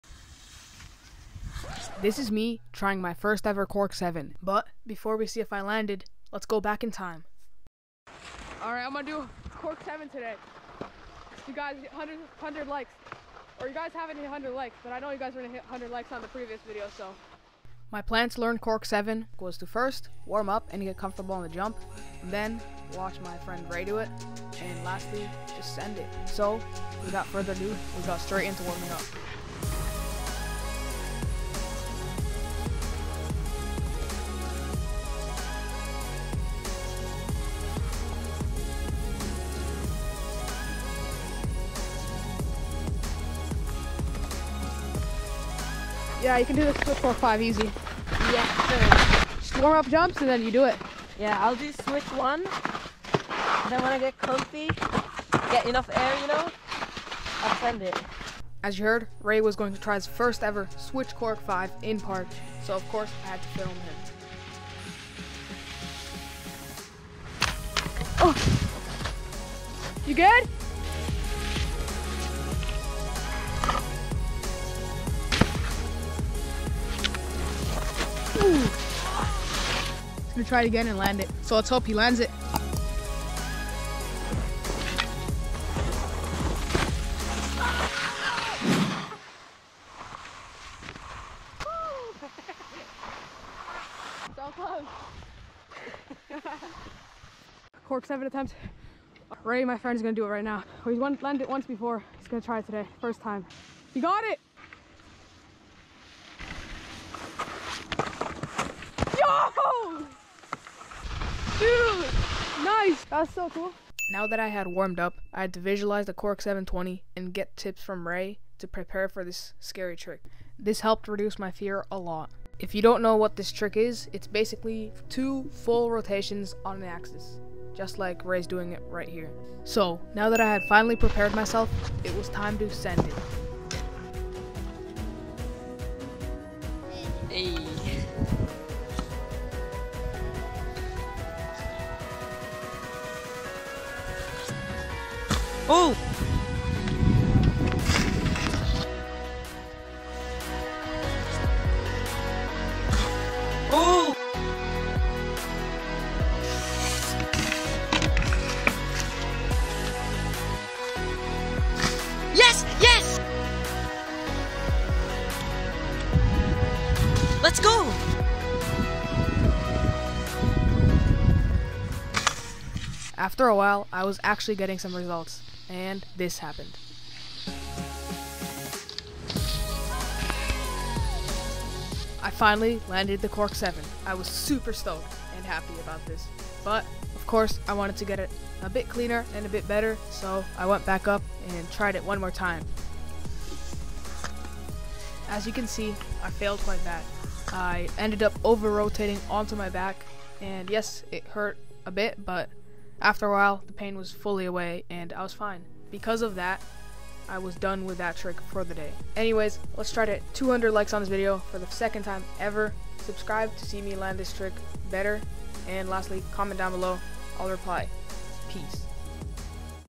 Today I learned to cork 720 on skis... Cork 720
Edited by Eetu Vierinen